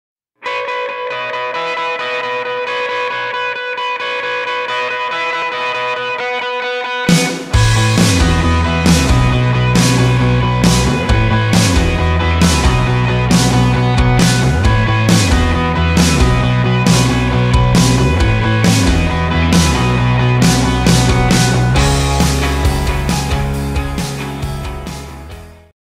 This is Tomorrow Newcastle Trailer
This is Tomorrow Festival Newcastle Trailer featuring Thirty Seconds to Mars.